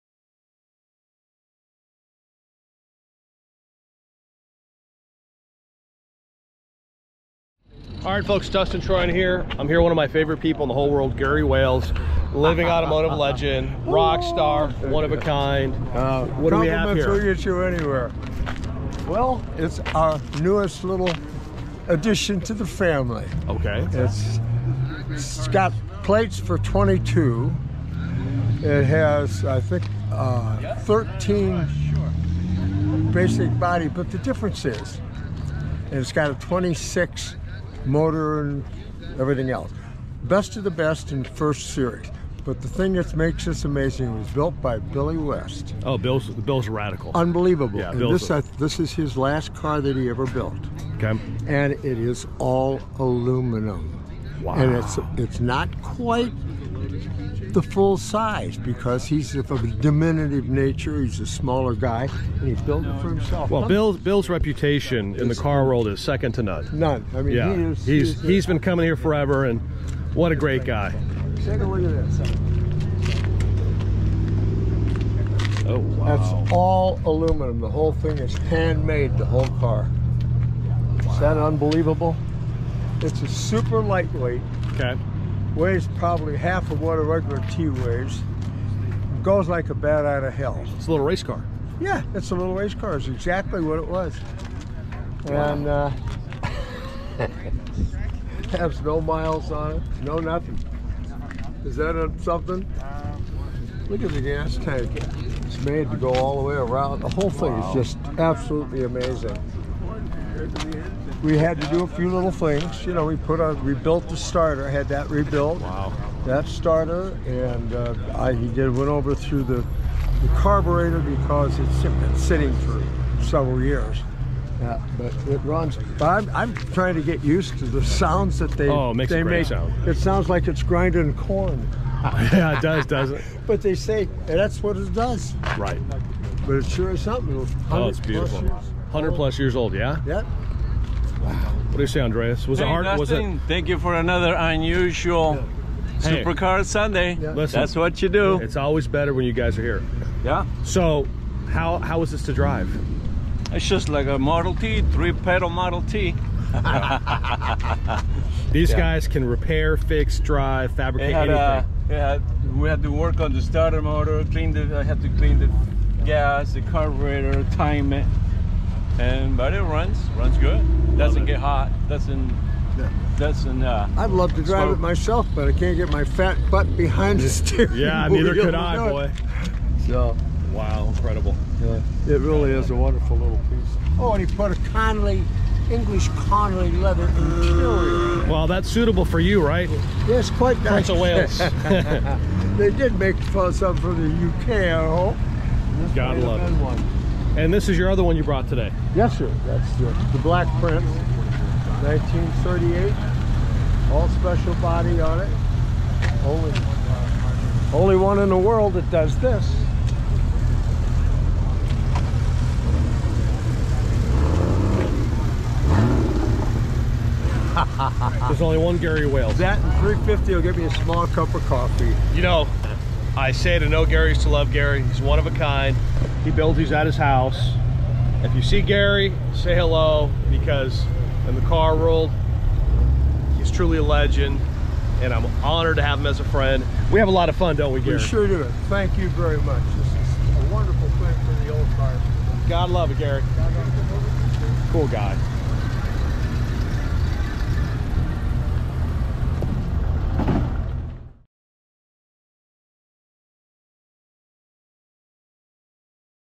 Gary Wales Added A New Car To The Collection | Built By Billy West | Two Automotive Legends!
Gary called me when he was going to pick up this amazing car at Bill West's  house.  Billy is very respected car builder and legend.  Gary brought it home and took a few days to clean it and get it up to speed and debuted it at Supercar Sunday.  Built by a legend and added to the collection of a legend.  It was a great day for all... enjoy the vid.